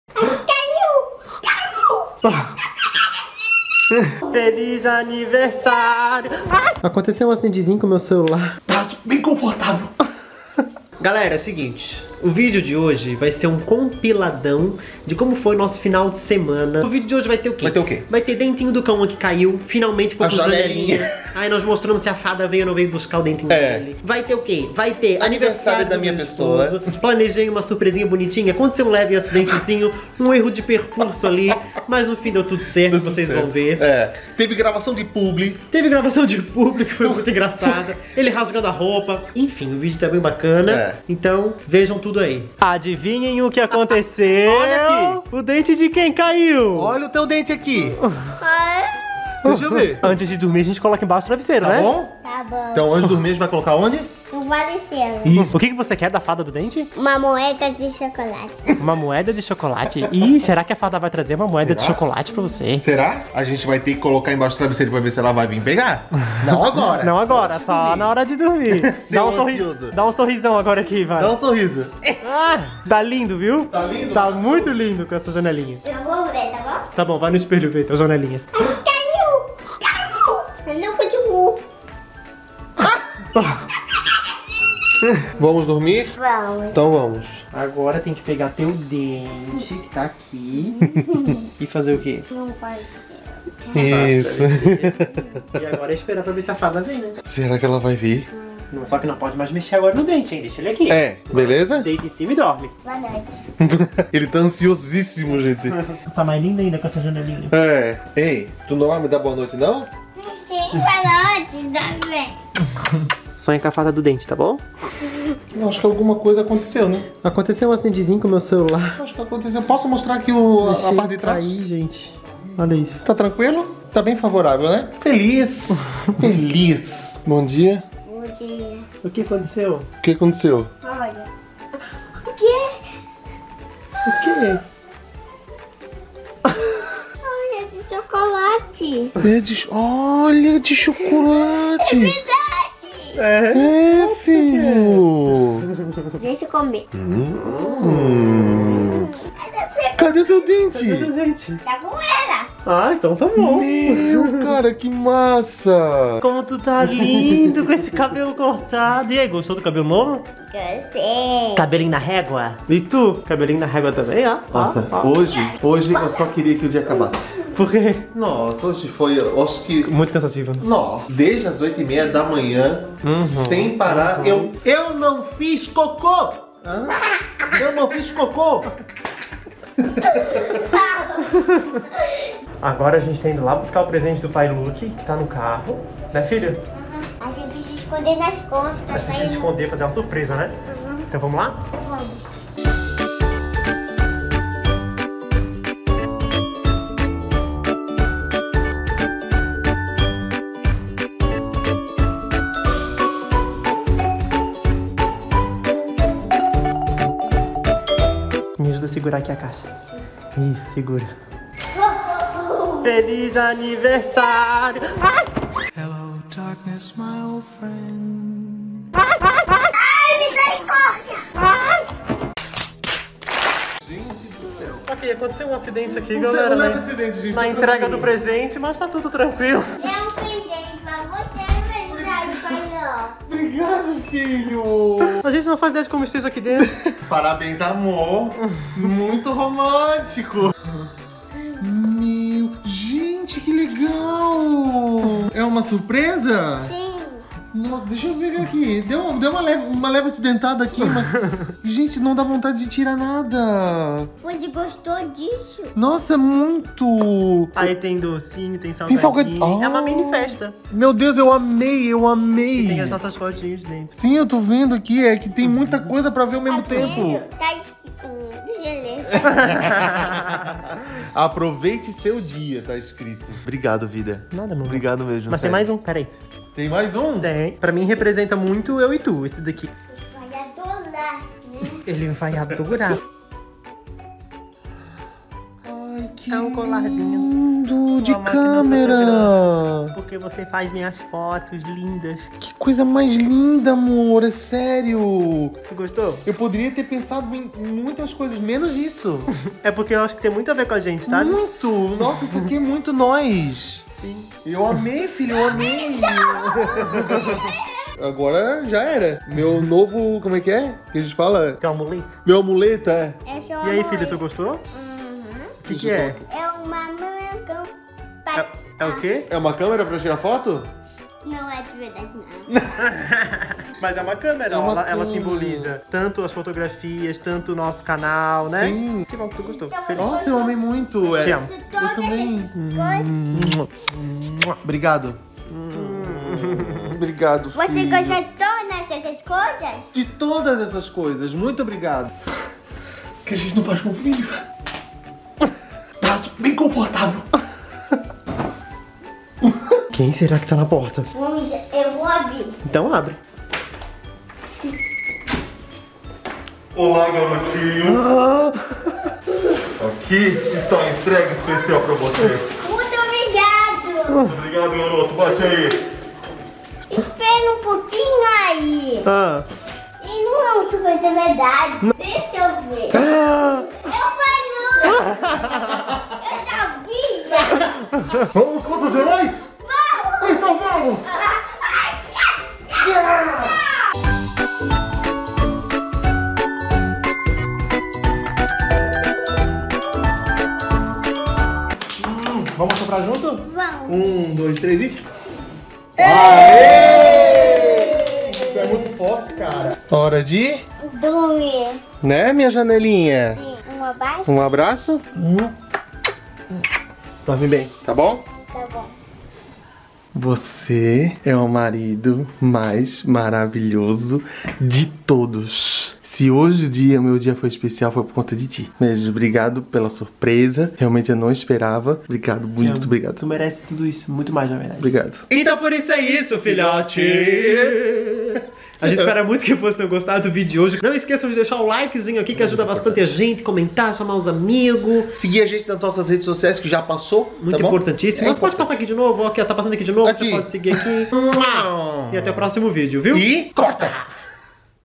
CAIU O DENTE DO KAUAN E SURPRESA PRO LUKE
No vídeo de hoje trazemos um compilado do que foi o nosso final de semana/feriadão. Tem dente do Kauan, surpresa do Luke e muito mais...